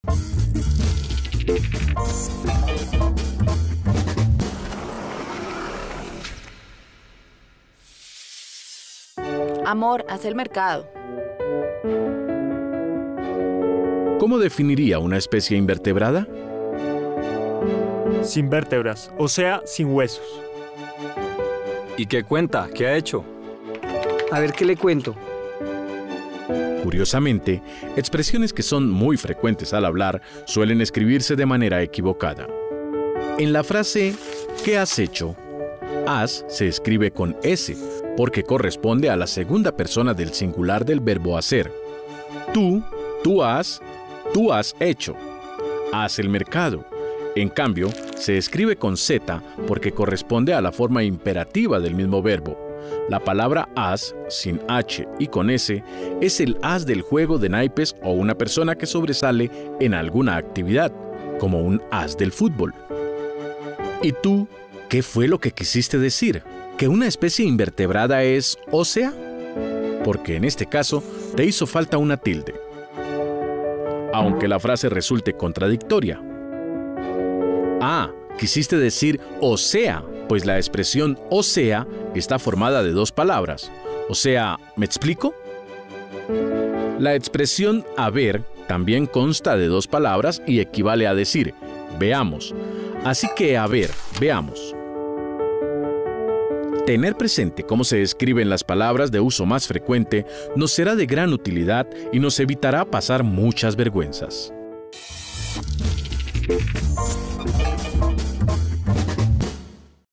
Exprésatebien "Haber, A Ver" - Universidad Nacional de Colombia
En español existen palabras que se escriben o pronuncian igual, lo cual hace que tengamos confusiones al momento de elegir la que queremos usar.